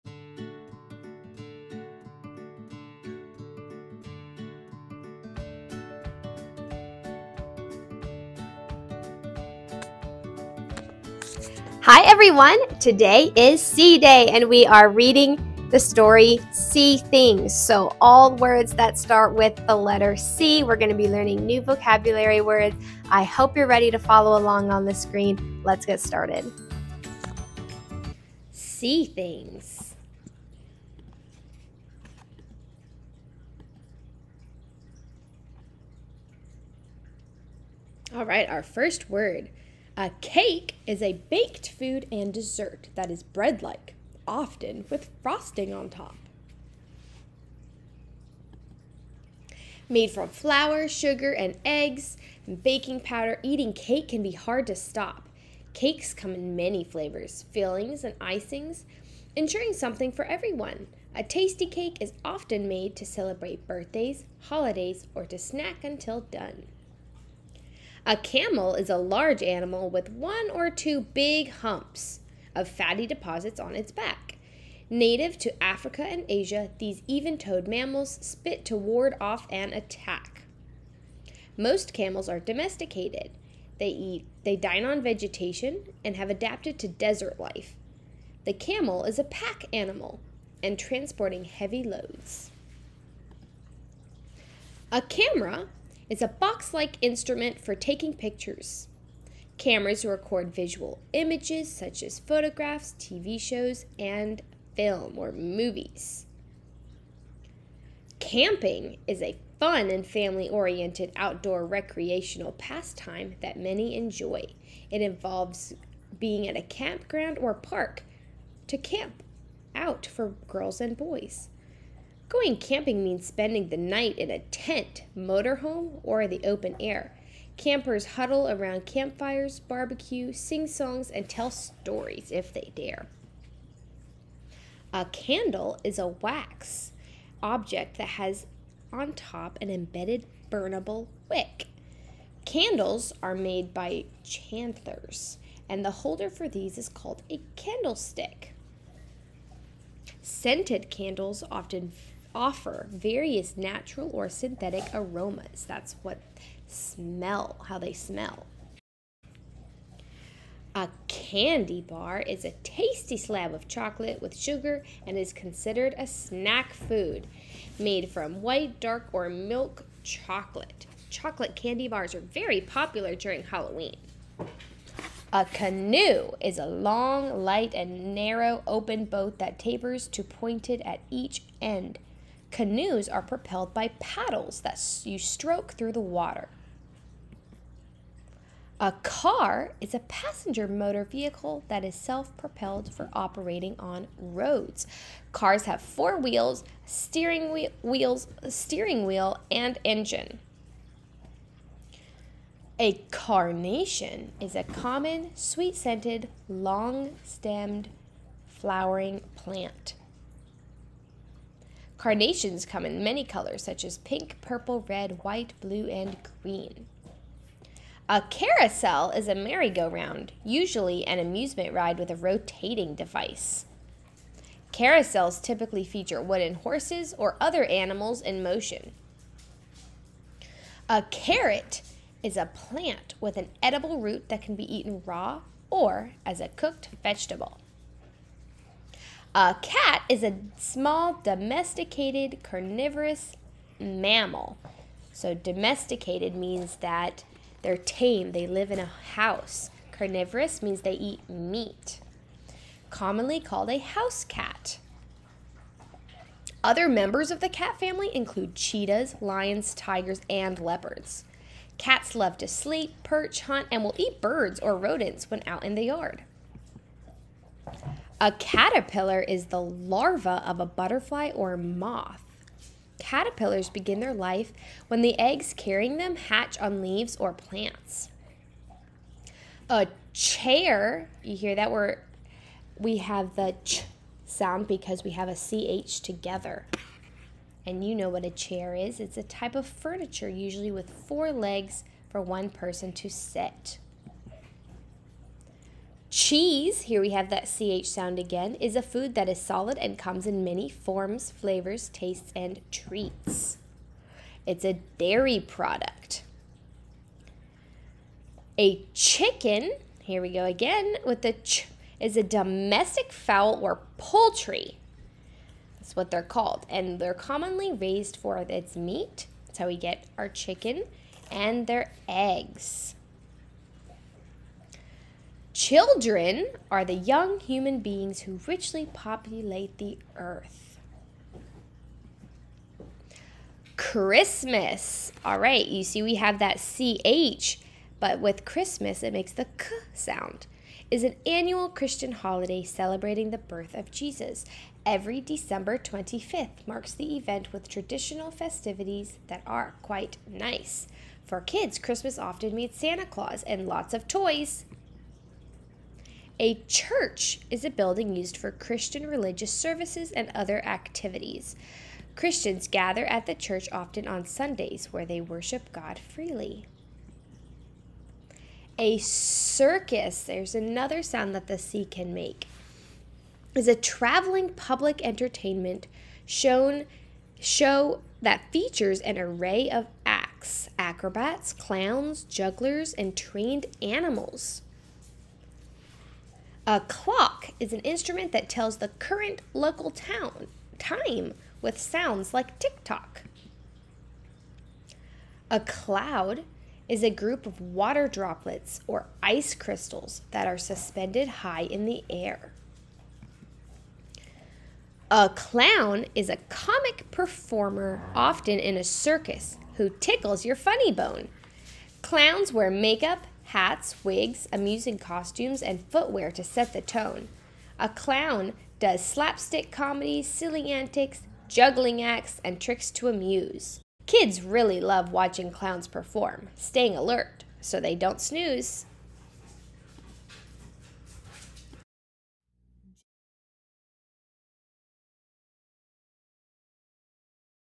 All About the Letter 'C'
We've gone through the alphabet and the letter sounds! Today we get into vocabulary words that all start with the different sounds of C while Mrs. B reads short little poems.